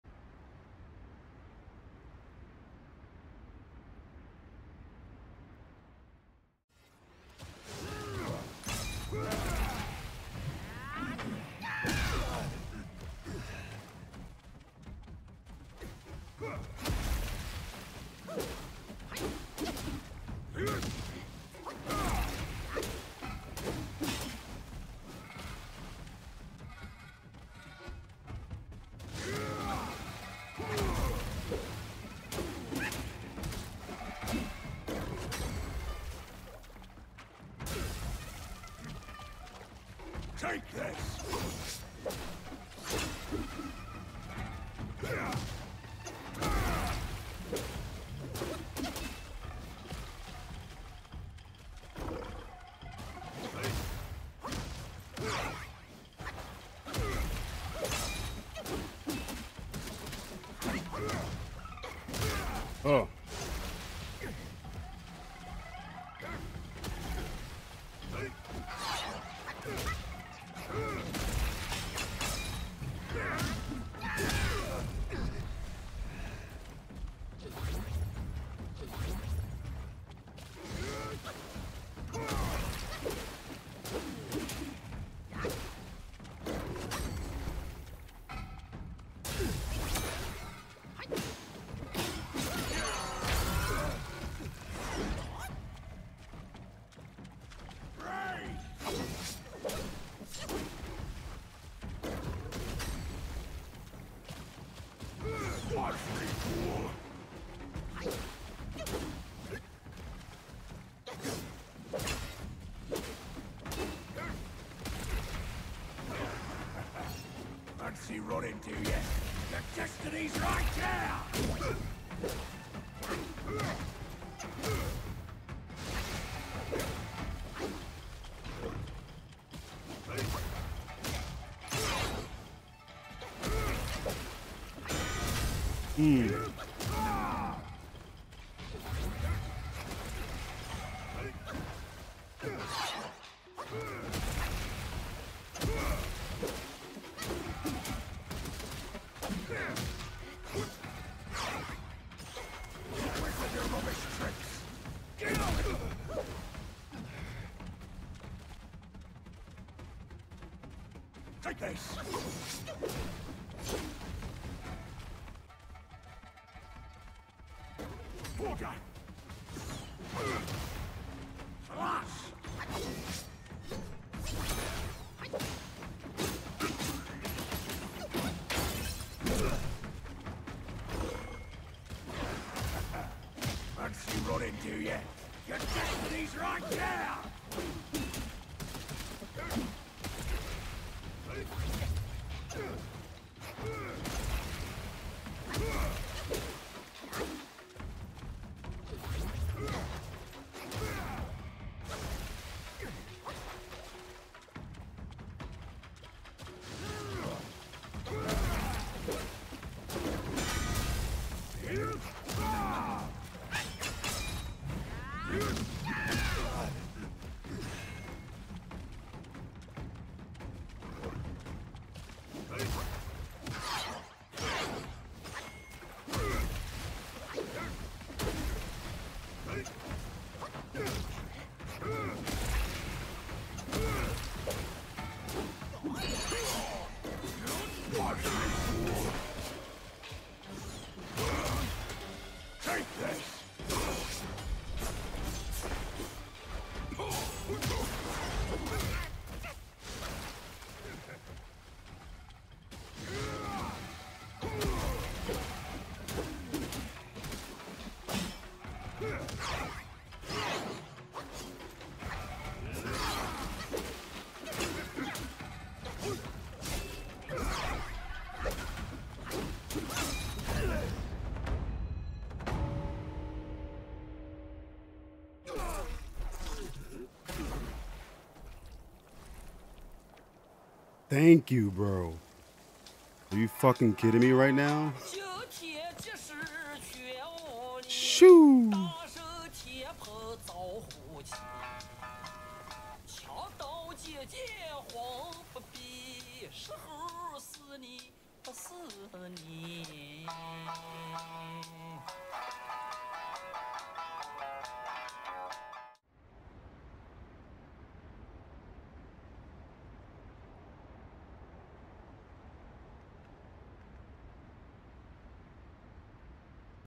Vanguard Tiger Finally Beaten.
A few minutes after streaming, I beat this damn tiger.